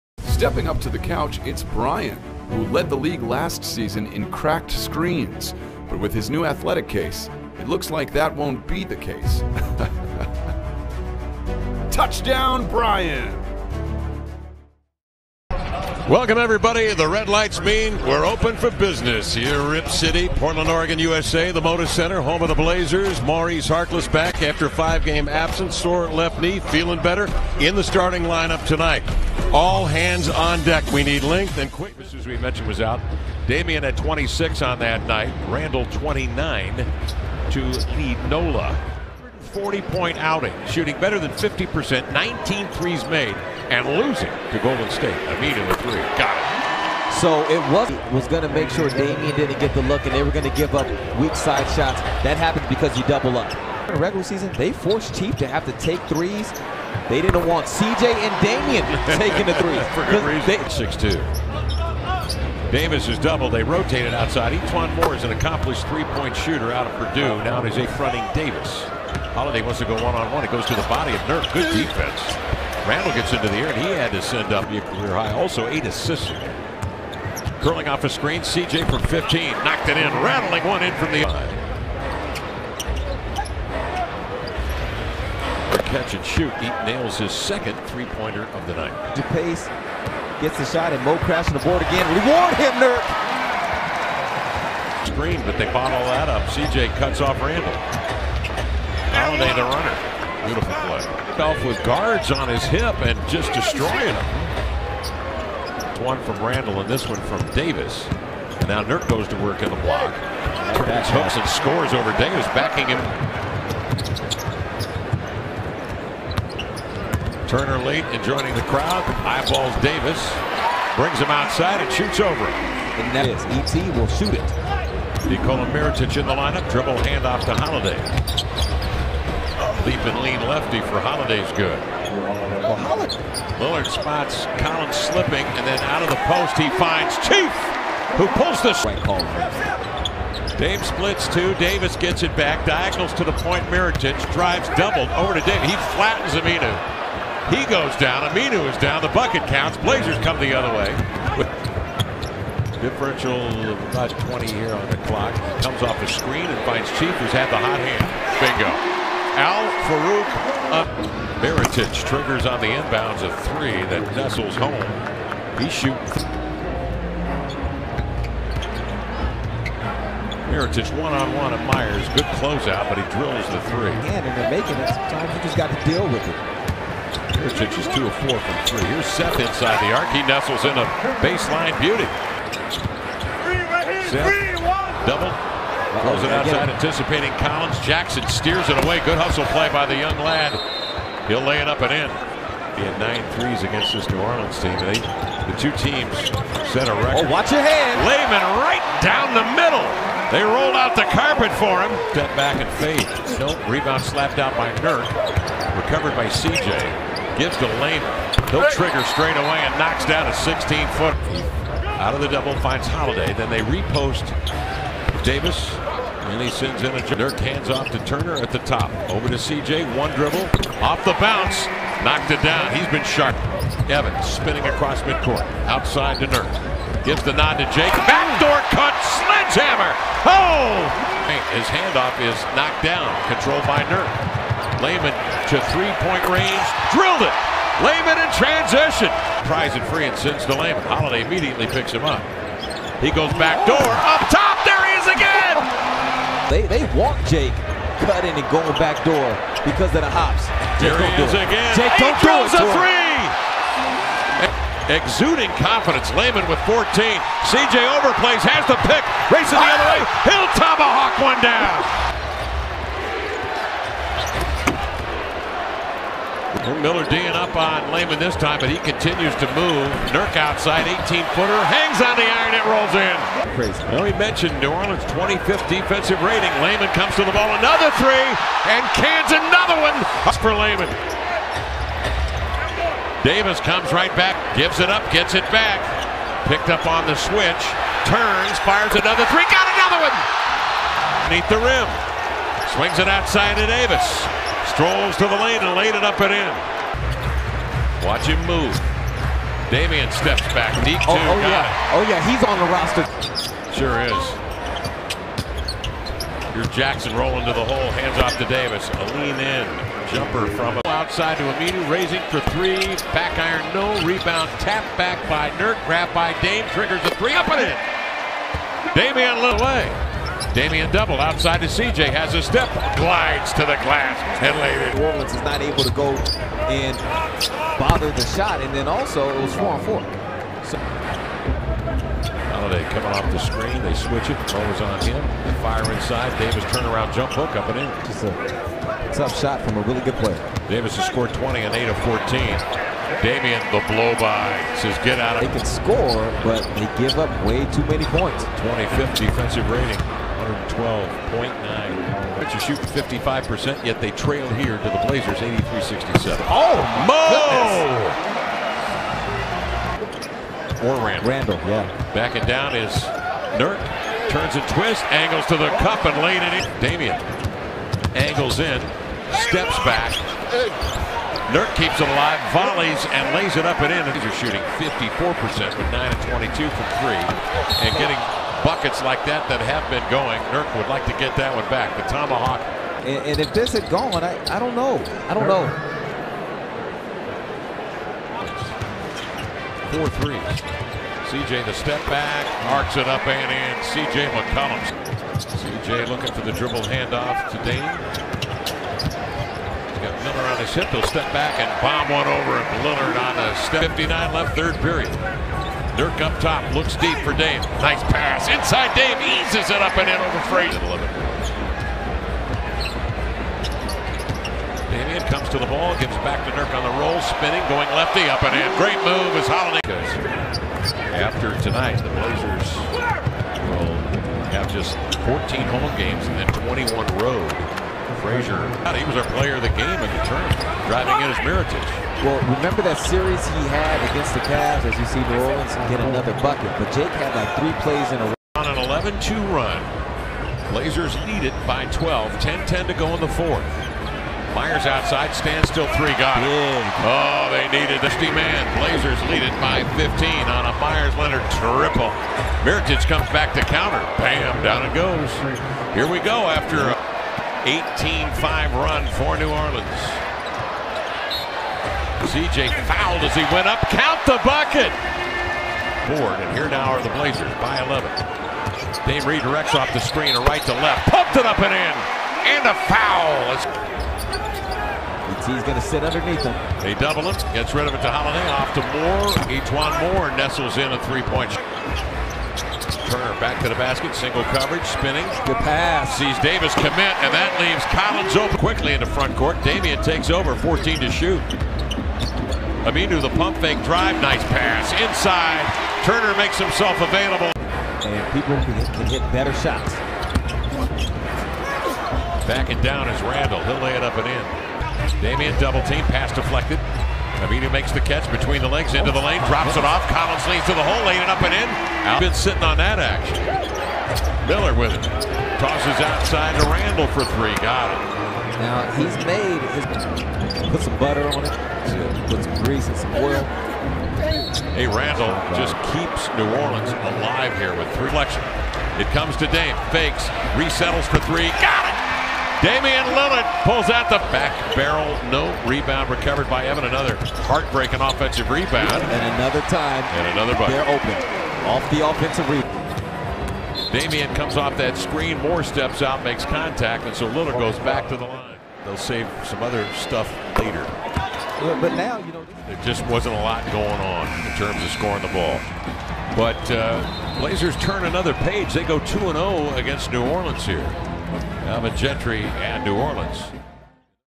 New Orleans Pelicans vs Blazers - Full Game Highlights | January 18, 2019 | 2018-19 NBA Season
Blazers  vs New Orleans Pelicans - Full Game Highlights | January 18, 2019 | 2018-19 NBA Season

for official scores & highlights.